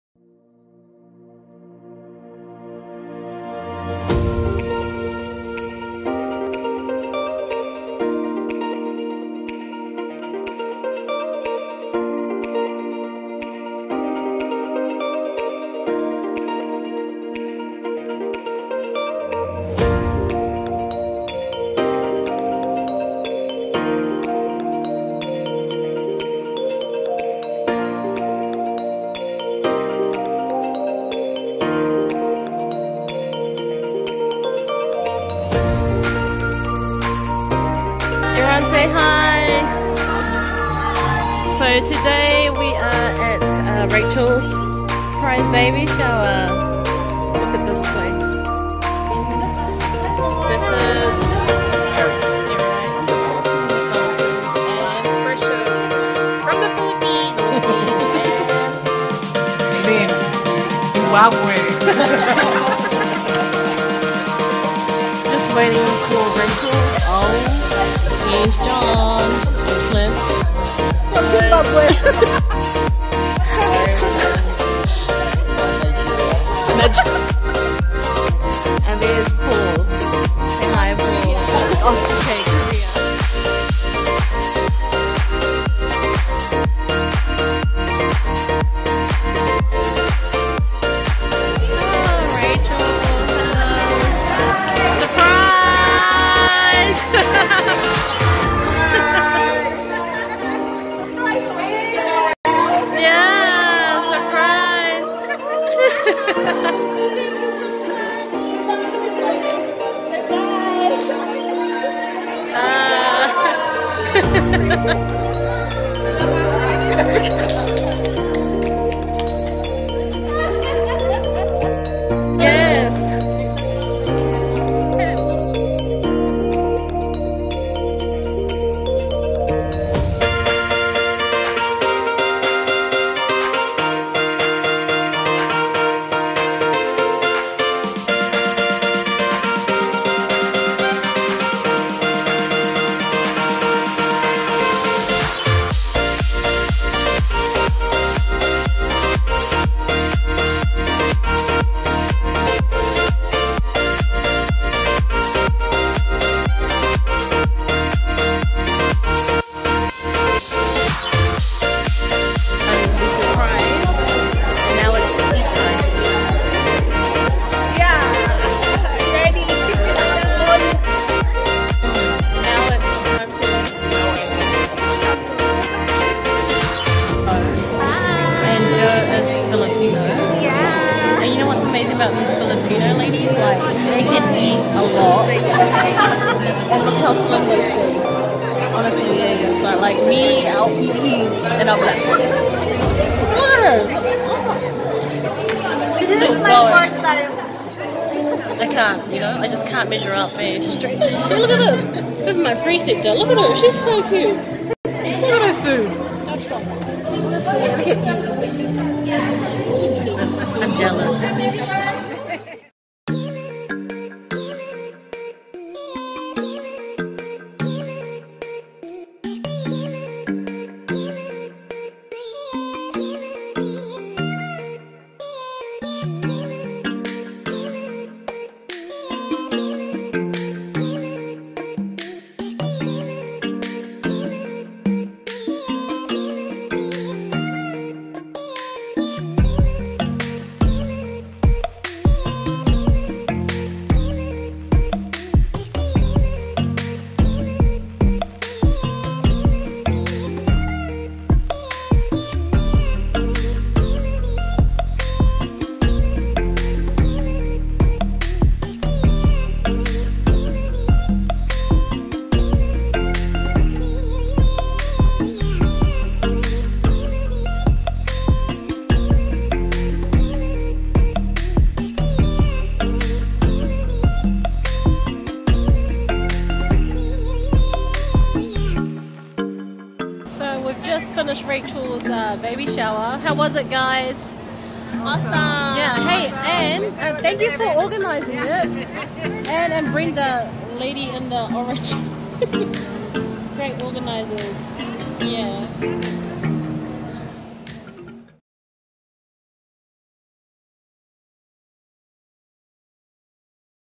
Surprise Baby Shower | Vlog 6
A snippet into Rachael's surprise baby shower.  Good vibes with great people